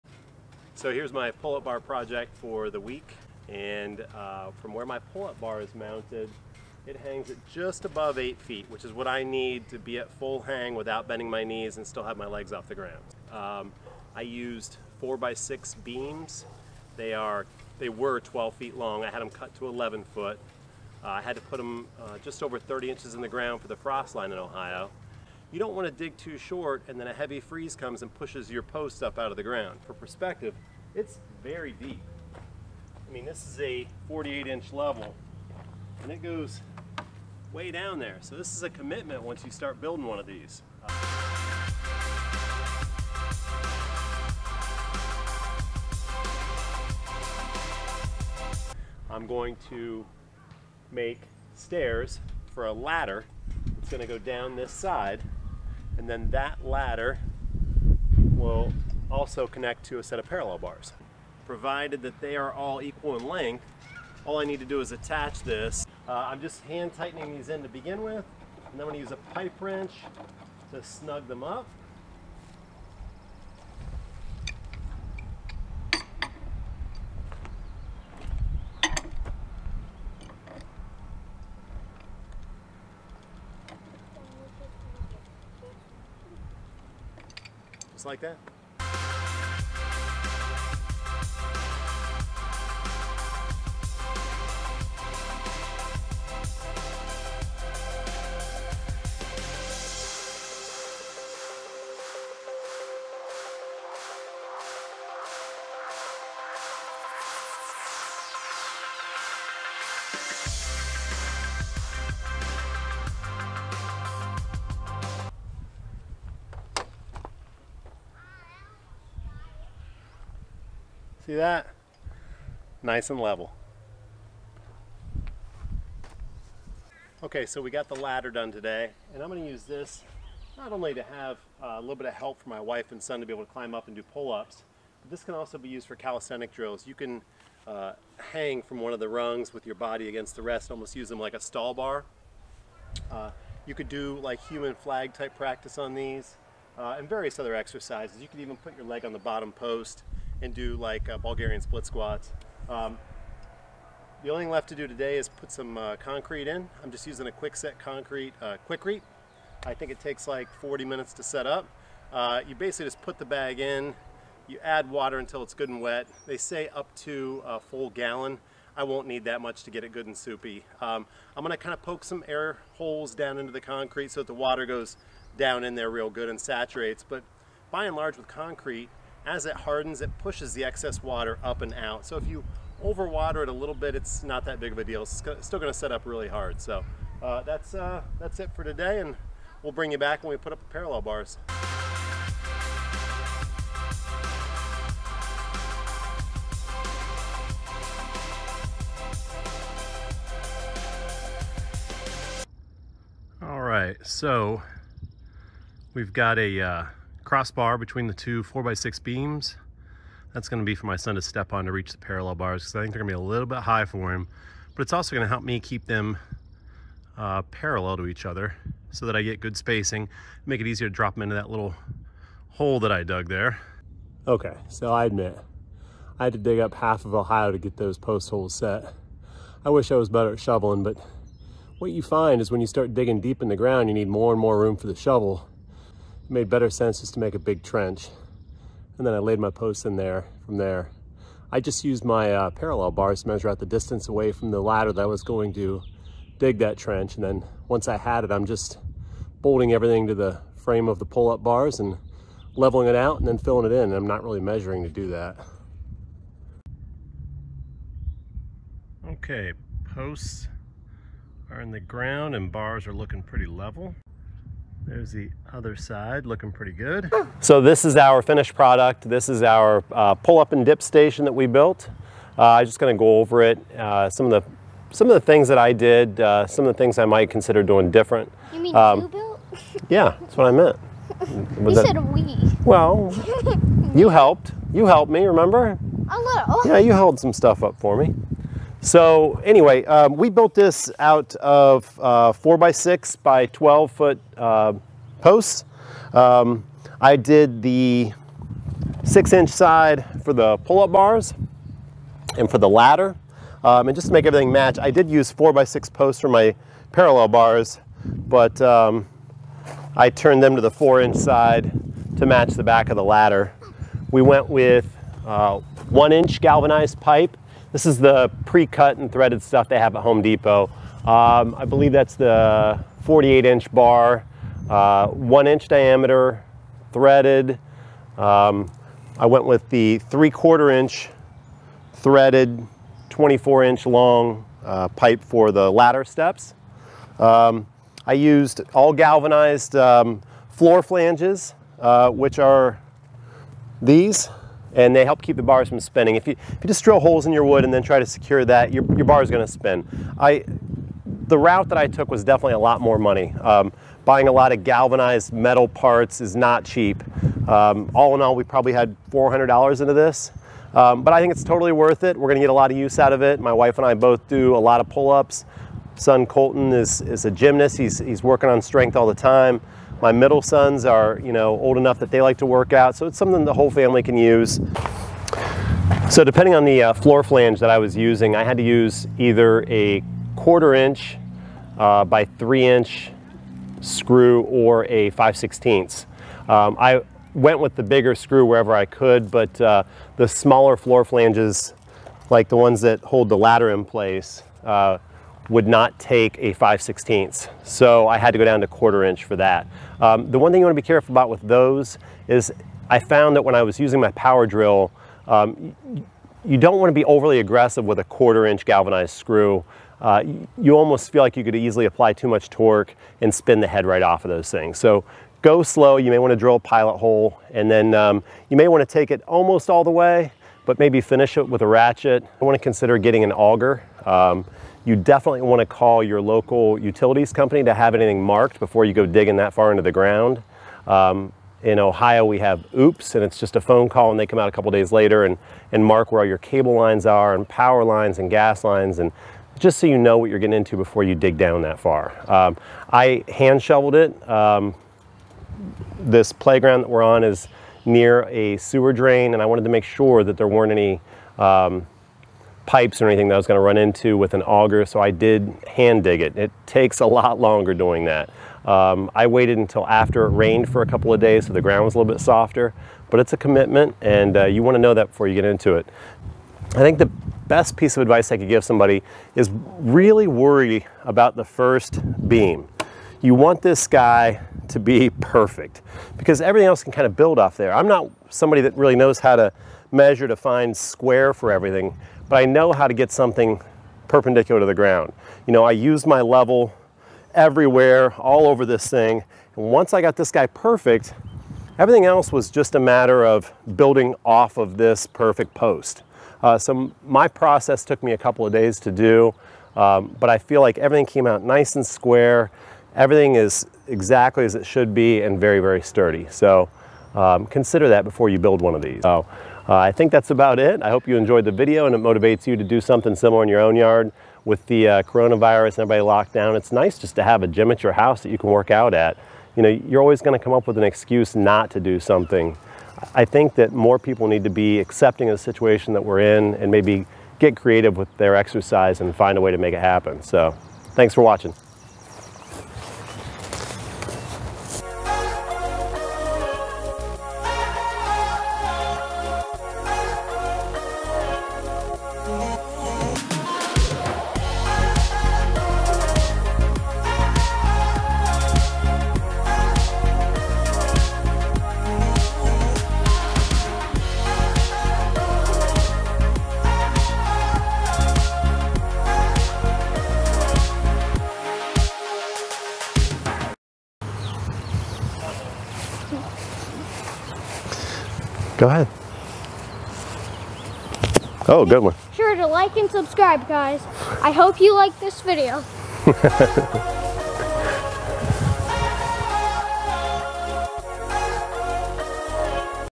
Pull Up Bar Build
In this video I cover the process I used for making a backyard calisthenics workout station. It features a 4 foot long pull up bar, 5 foot long parallel bars and a ladder that can be used as a Swedish Ladder. The entire unit is extremely sturdy and can be used for many different exercises. You can buy everything you need from Home Depot and you need very little no how to be able to do this yourself.
Parts I Used:

(4) 4' x 6' x 12' Pressure Treated Lumber Posts
I had 3 of these cut to 11' each and 1 cut in half
(4) Galvenized Floor 1" Flanges (for pull up and parallel bars)
(14) Galvenized Floor 3/4" Flanges (for the ladder)
(1) 4' Long (Threaded) Galvenized 1" diameter pipe
(2) 5' Long (Threaded) Galvenized 1" diameter pipes
(24) 5/16" Galvenized 3" long screws (for the 1" flanges)
(56) 1/4" Galvenized 3" long screws (for the 3/4" flanges on the ladder)
(6) 50 lb bags of Quikrete (I just used one bag in for each post I set)